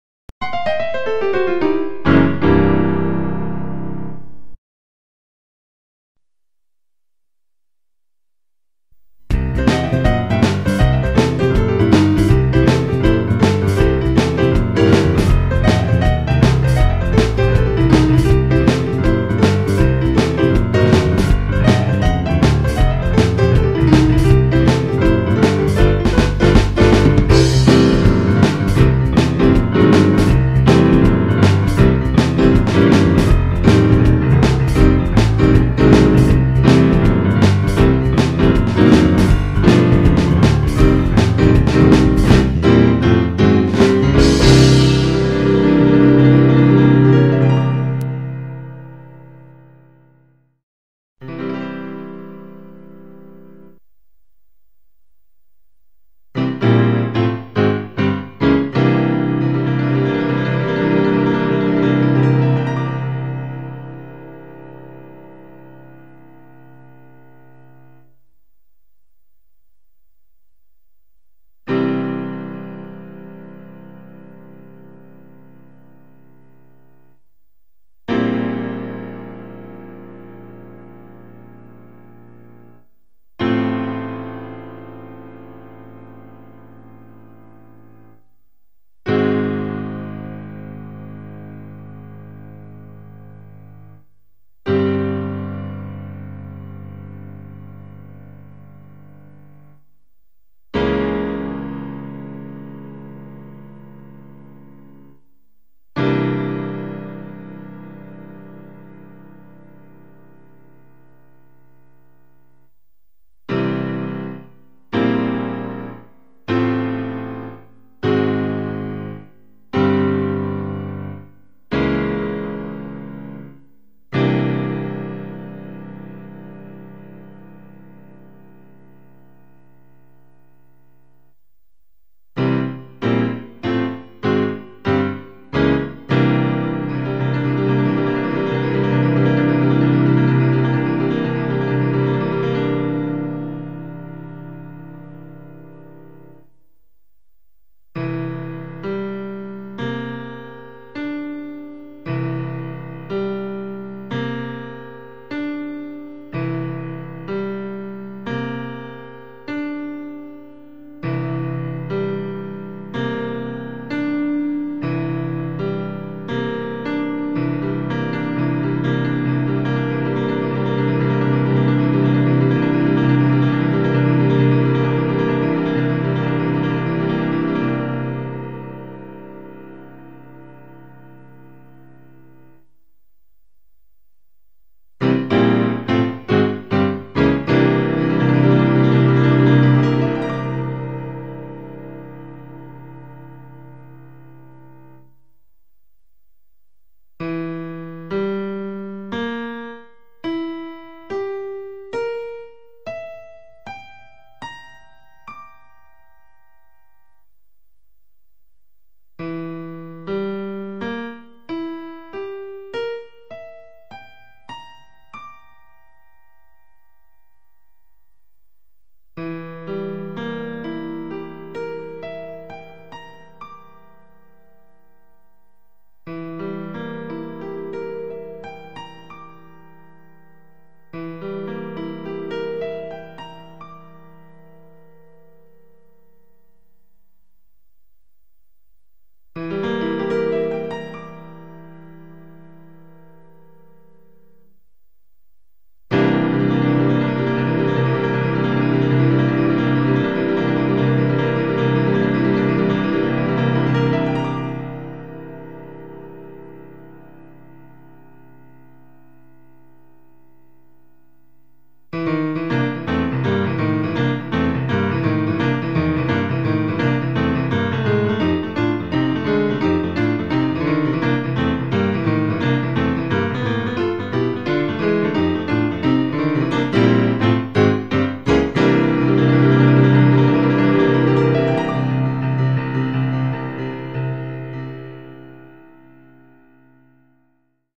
When you want to END in STYLE ! BOOGIE WOOGIE Honky Tonk Piano ! Awesome Blues Tutorial Lesson
Try this Amazing ending riff to finish off in style...